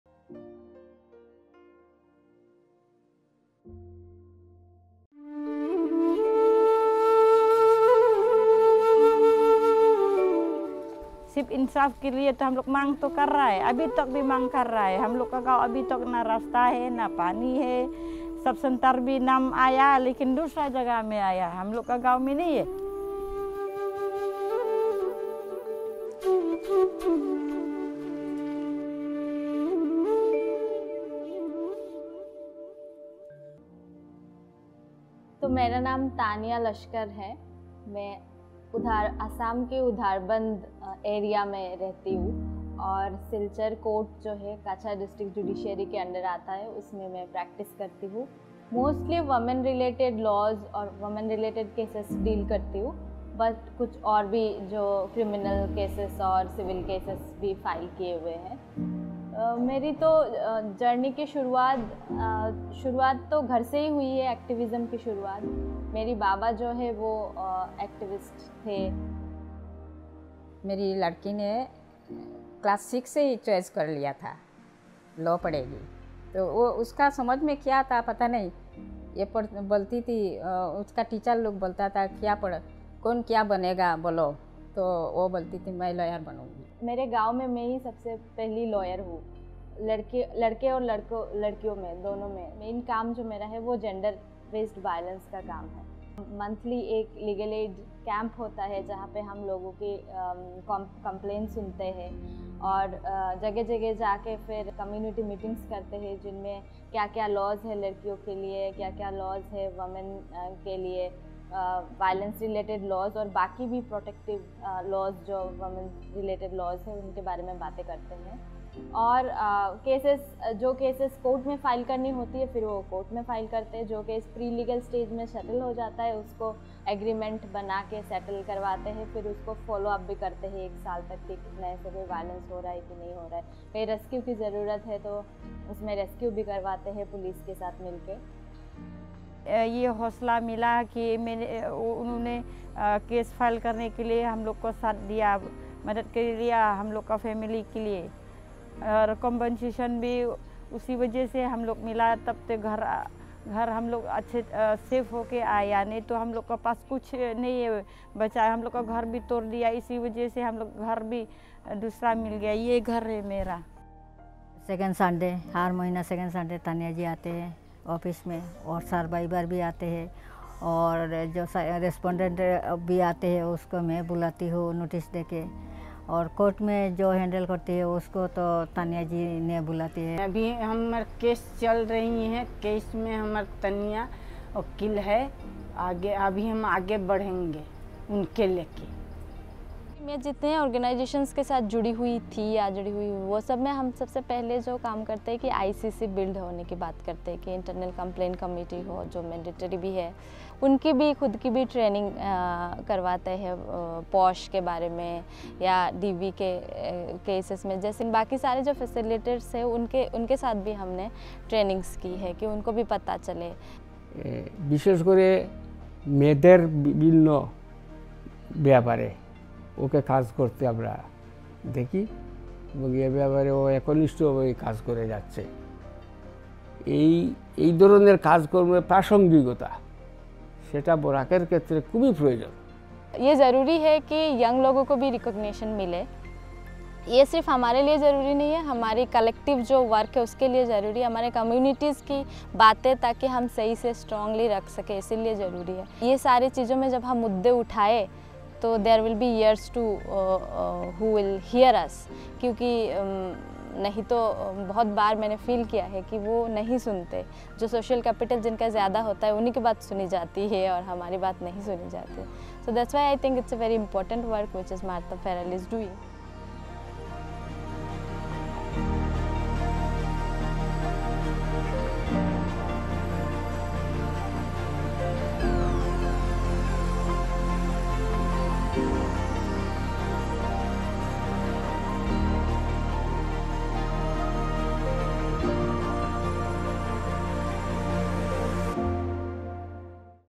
8th Martha Farrell Award 2024 | Winner | Special Jury Award (Individual) | Taniya Laskar
A feminist lawyer from Barak Valley, Assam, Taniya provides legal support to women from minority communities, and is working to make access to justice easier for the people through documentation, legal interventions, legal research and building legal literacy in marginalised communities.

Taniya has worked on issues of gender-based violence and discrimination, providing legal aid and support to survivors of violence. More recently, she has also been working extensively on the National Register of Citizens (NRC), documenting the impact of the NRC on individuals in the region and debunking misconceptions around the NRC in communities. She is also a co-founer of Sexuality and Gender Equality (SAGE), a queer-rights organisation advocating for LGBTQI+ issues and sensitisation towards the LGBTQI+ community.

असम के बराक घाटी से एक नारीवादी वकील, तान्या अल्पसंख्यक समुदायों की महिलाओं को कानूनी समर्थन प्रदान करती हैं और दस्तावेजीकरण, कानूनी हस्तक्षेप, कानूनी शोध और हाशिए पर स्थित समुदायों में कानूनी साक्षरता के निर्माण के माध्यम से न्याय तक उनकी पहुँच को आसान बनाने के लिए काम कर रही हैं।

तान्या ने लिंग आधारित हिंसा और भेदभाव के मुद्दों पर काम किया है, और हिंसा से बचे लोगों को कानूनी सहायता और समर्थन प्रदान किया है। हाल ही में, वह राष्ट्रीय नागरिक रजिस्टर (एनआरसी) पर भी बड़े पैमाने पर काम कर रही हैं, क्षेत्र में व्यक्तियों पर एनआरसी के प्रभाव का दस्तावेजीकरण कर रही हैं और समुदायों में एनआरसी के बारे में गलत धारणाओं को दूर कर रही हैं। उन्होंने पूर्वोत्तर भारत में एलजीबीटीक्यू व्यक्तियों के अधिकारों और समानता की वकालत करने के लिए सपोर्टिंग एंड एडवोकेटिंग फॉर जेंडर इक्वेलिटी (SAGE) की सह-स्थापना भी की है।